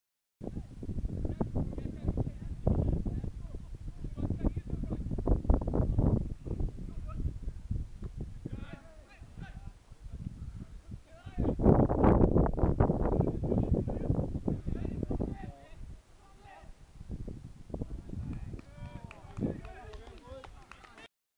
EAST CORNWALL PREMIER LEAGUE GAME, ST MINVER 7-1 ST STEPHEN 1st team.
Match highlights,Craig Coad goes close for St Stephen early on in the 1st half.